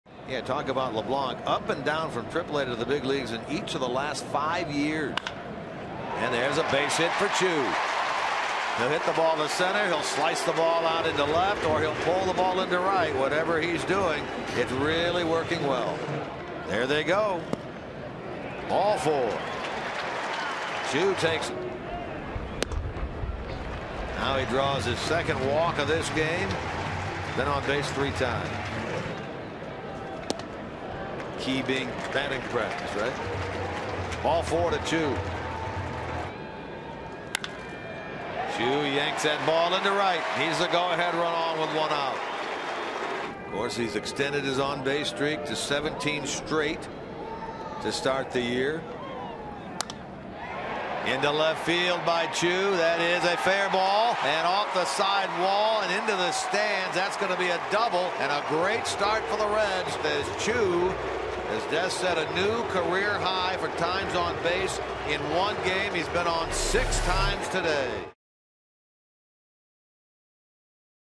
Choo reaches base six times
4/20/13: Shin-Soo Choo reaches base a career-high six times with three hits and three walks